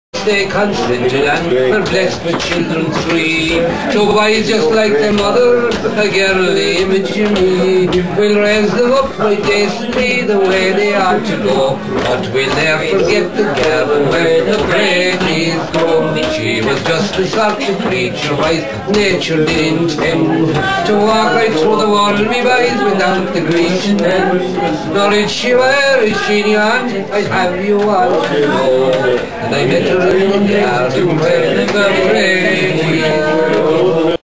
Jim
"Jim of the Hill" singing in his "kitchen" tavern in County Tipperary, Ireland